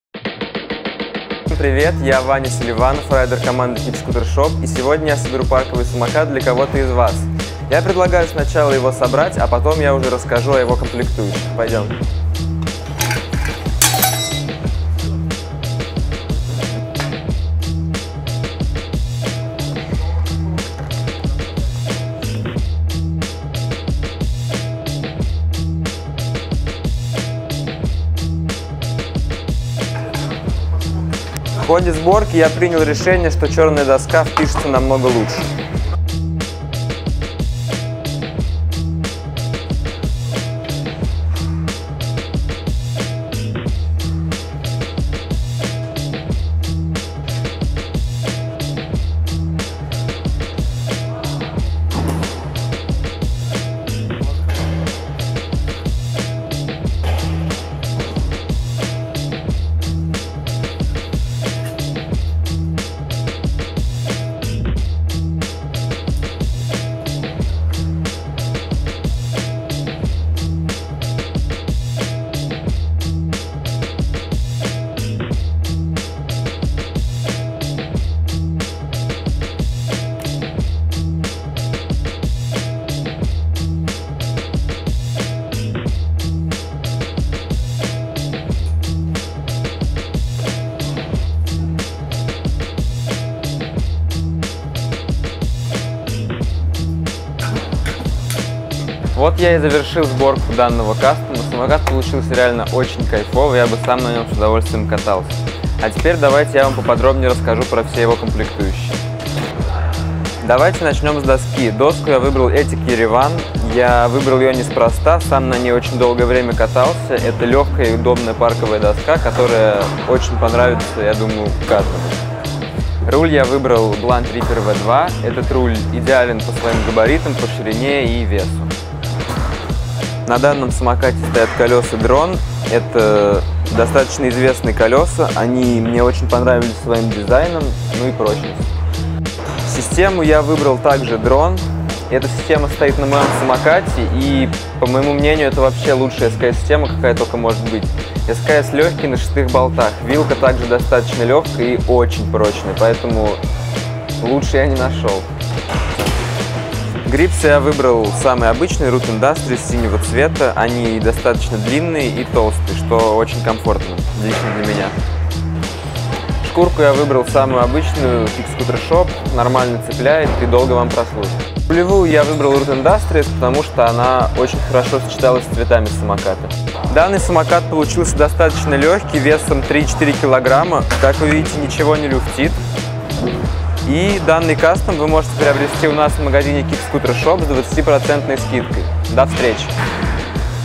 Кастом Чемпиона | KICKSCOOTERSHOP
Представляем кастом от Ивана Селиванова, действующего Чемпиона России!
Кастом настроен на прогресс.
Все запчасти данного кастома Ваня тестировал сам лично и собирал его буквально "под себя"!

У вас есть уникальная возможность приобрести «Кастом от Селиванова» со скидкой в 20% в нашем магазине KICKSCOOTERSHOP!

Итоговая цена 27 000₽

Съемка и монтаж: Артем Степченков